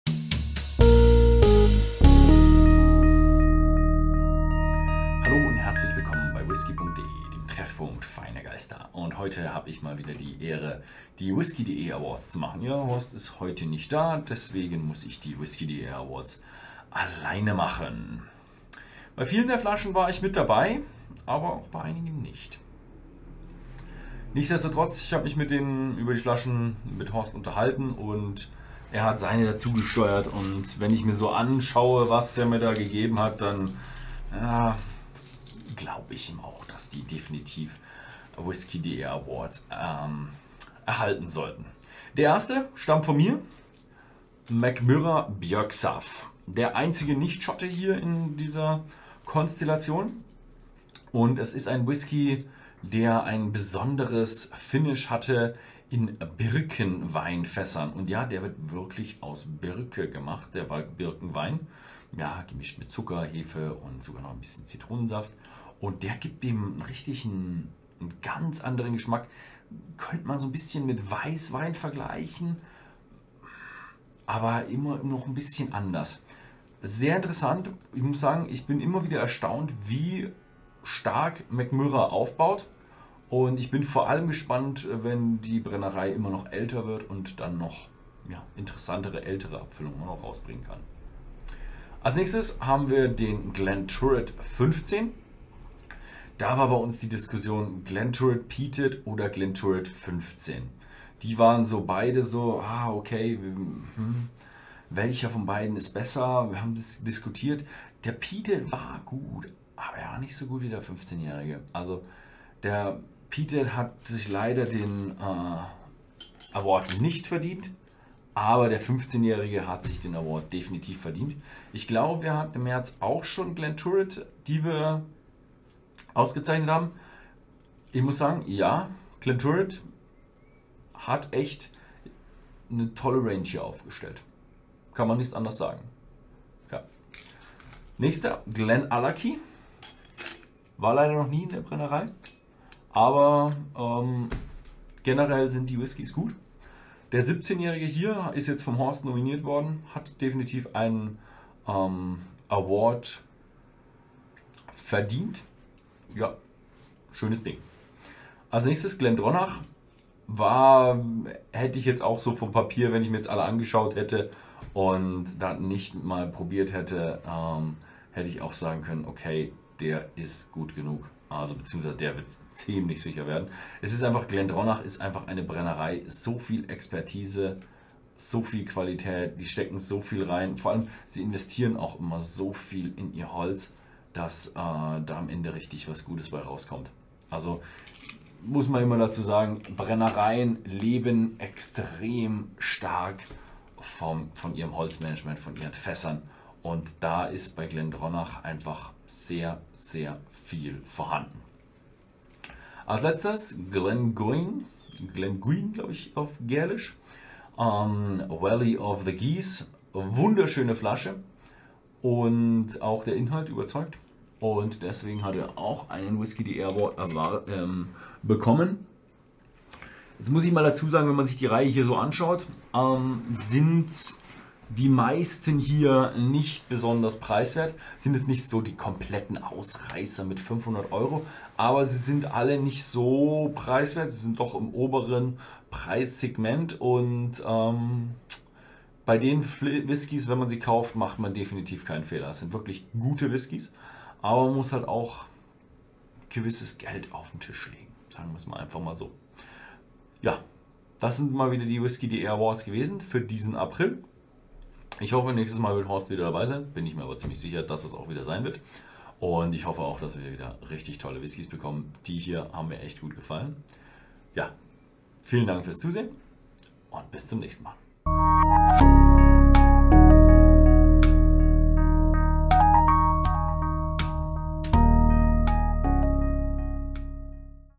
Whisky.de Award April 2021
Ben und Horst Lüning stellen die besten Whiskys des Monats April 2021 vor. Die Auswahl wird aus allen Whiskys getroffen, die von Ben und Horst Lüning im Monat April verkostet wurden.

00:00​ Whisky.de Awards
00:40 Mackmyra Björksav
01:32 Glenturret 15 Jahre
02:18​ Craigellachie 17 Jahre
02:38 Glendronach Cask Strength Batch 9
03:22 Glengoyne 18 Jahre